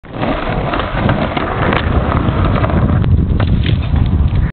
funny shit
i stack it trying to ride a skateboard